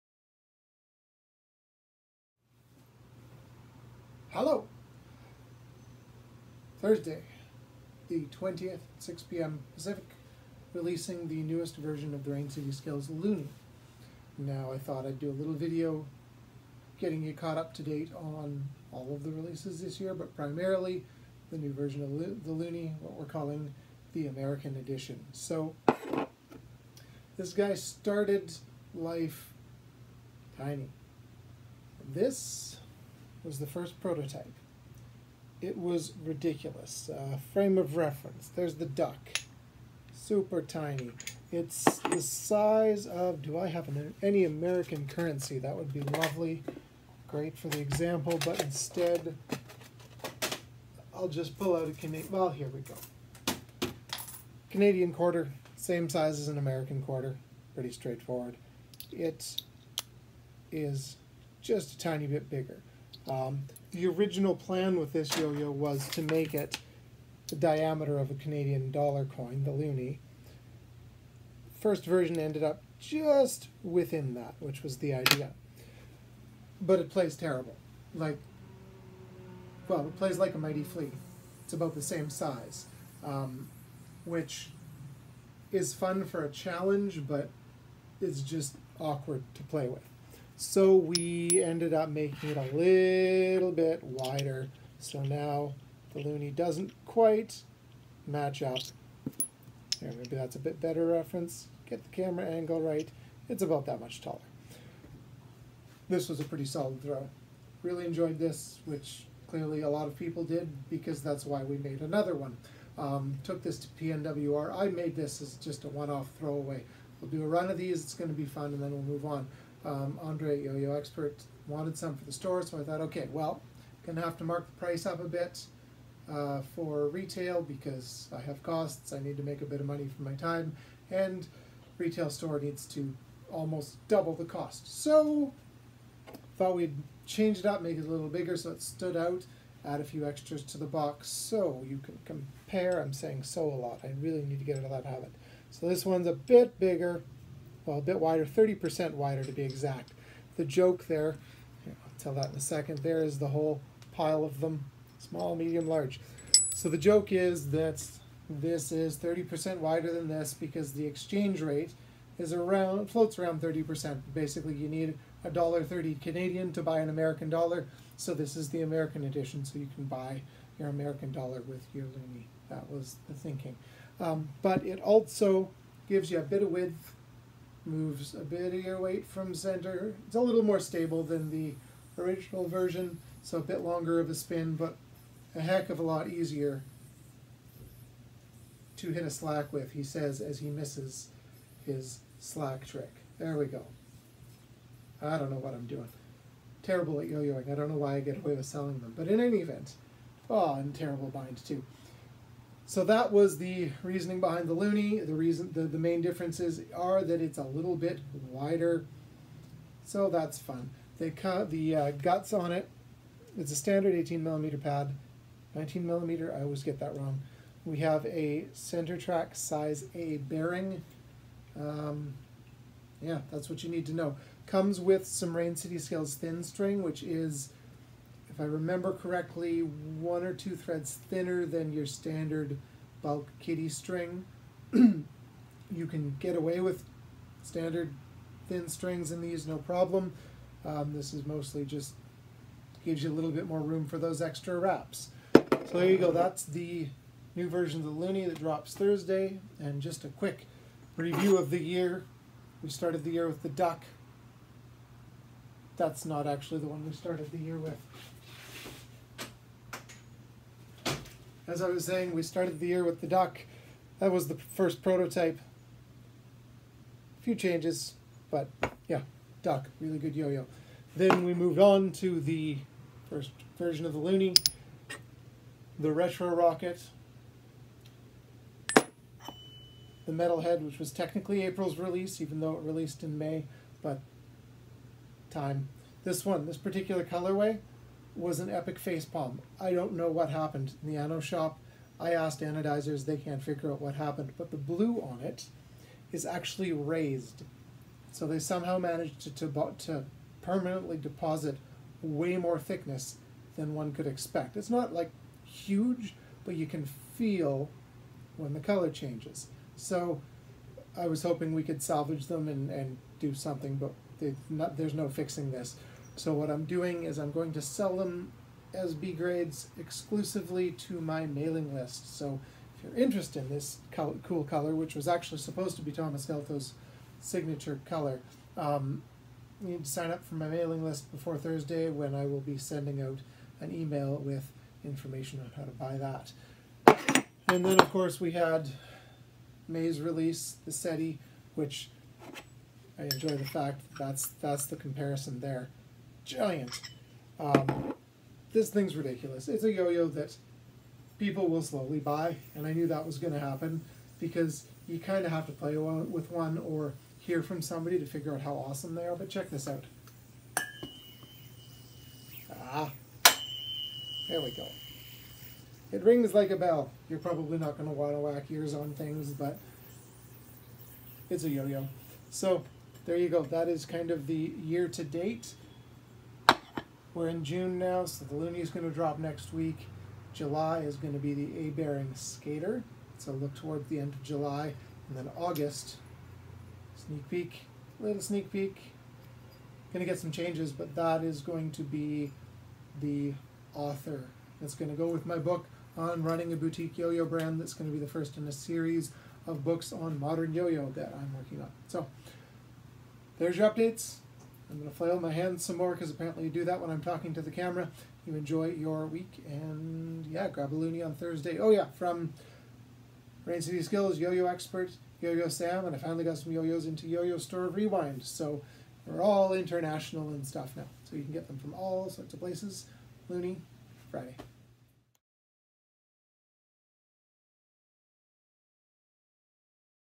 Jeremy's Yoyo Rant #11 - The evolution of the Rain City Skills "Loonie"
The American Edition of the Loonie drops Thursday at 6:PM Pacific at the following retailers:

Specs:
Material - Brass
Width - 33.1mm
Diameter - 31.8mm
Weight - 64.3g
Bearing - Size A Center flat track
Price - $50